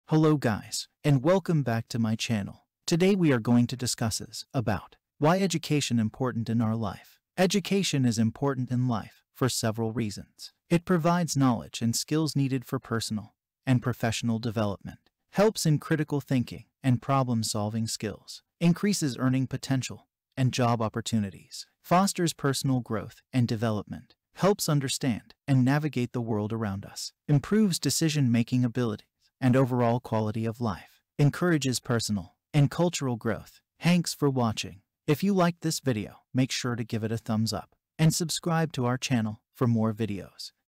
Why Education Important In Our Life
Education is important in life for several reasons:

It provides knowledge and skills needed for personal and professional development.

Helps in critical thinking and problem-solving skills.

Increases earning potential and job opportunities.

Fosters personal growth and development.

Helps understand and navigate the world around us.

Improves decision-making abilities and overall quality of life.

Encourages personal and cultural growth.

so you can receive more information videos like this.